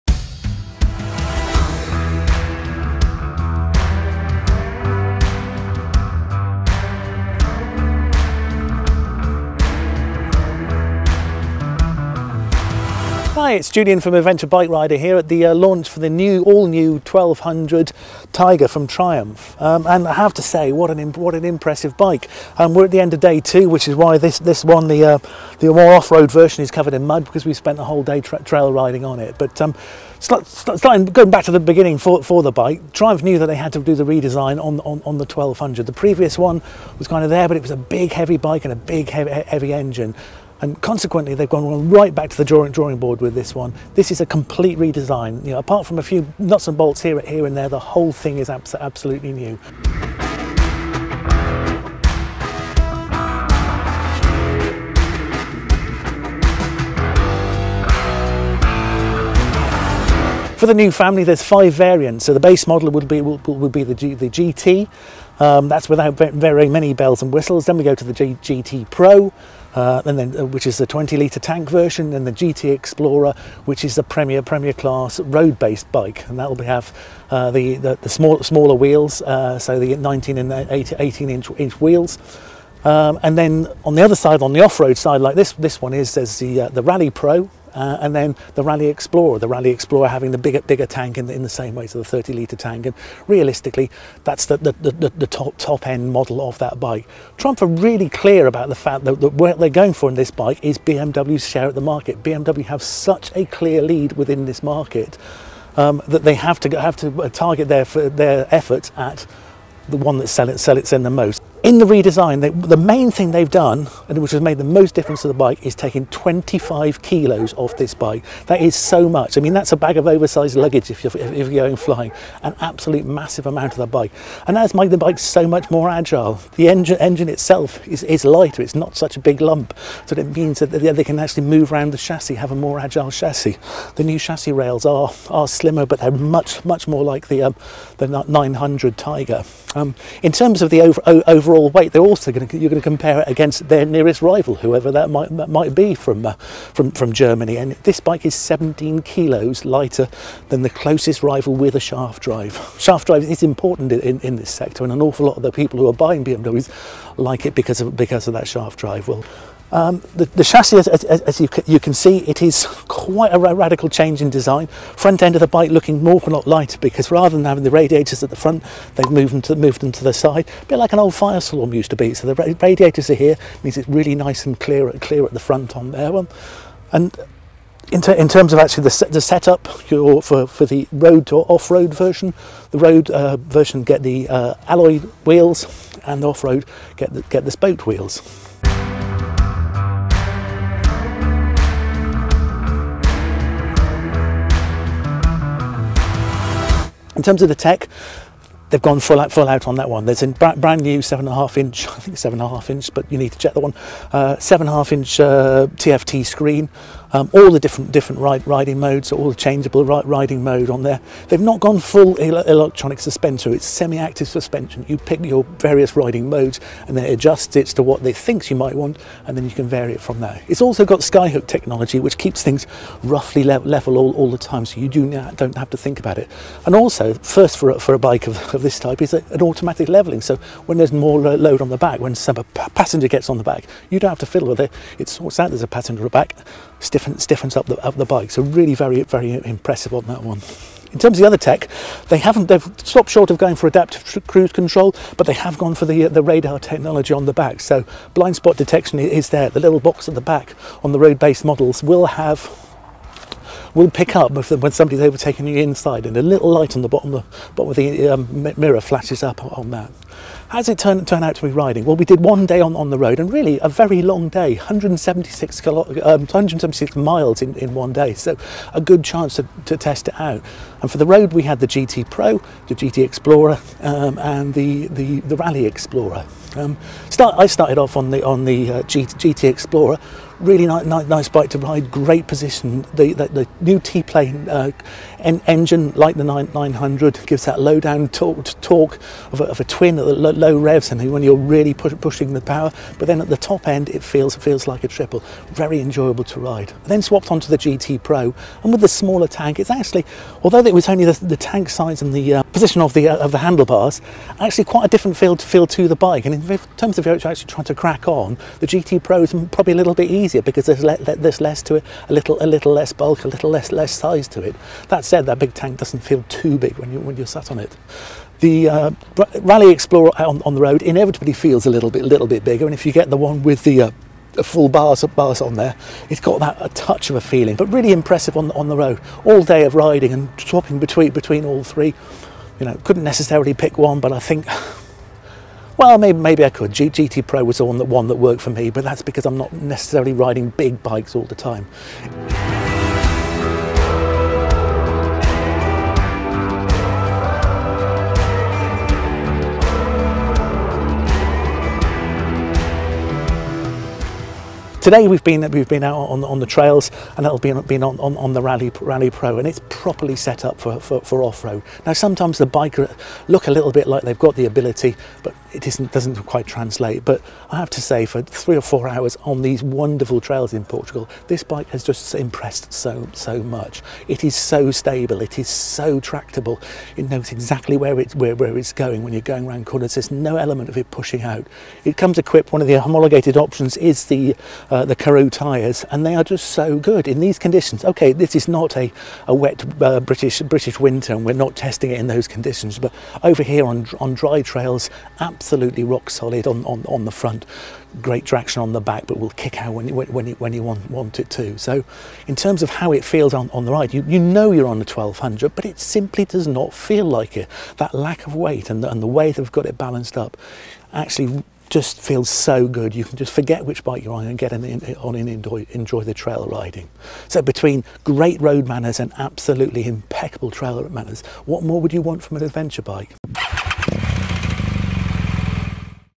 2022 Triumph Tiger 1200 Review
Triumph Tiger 1200 bike launch review. Join Adventure Bike Rider's chief bike tester Julian Challis as he travels to Portugal for the international launch of the new-for-2022 Triumph Tiger 1200.

Julian spent two days test riding the new Tiger 1200 both on and off road along twisting Portuguese roads and trails to find out if Triumph has managed to make an adventure bike capable of taking on the BMW R 1250 GS.

For 2022, the Triumph Tiger 1200 has been redesigned from the ground up and now features a selection of road and off-road focused models. All of them of lighter, leaner, and more technologically advanced than their predecessor.

Join Julian as he gives his first impressions of the new motorcycle. You can read his full review in the May/June 2022 issue of Adventure Bike Rider magazine.